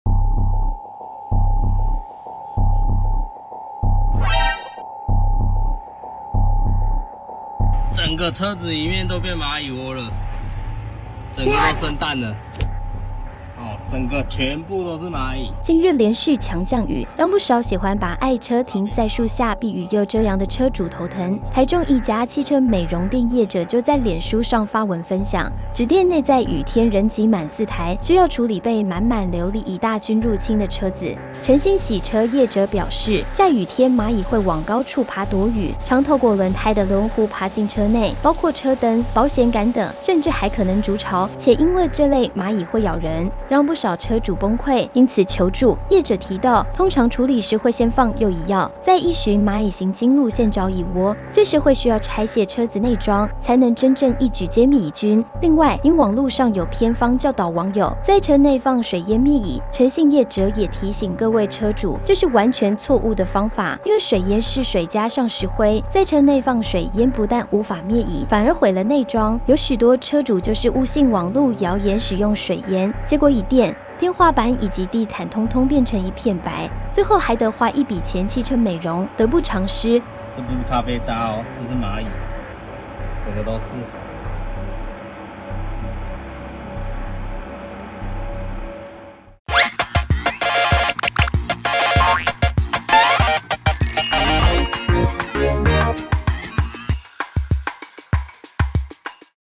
愛車變螞蟻窩😱雨天停樹下遭琉璃蟻入侵｜螞蟻
☑影片授權：陳小白 提供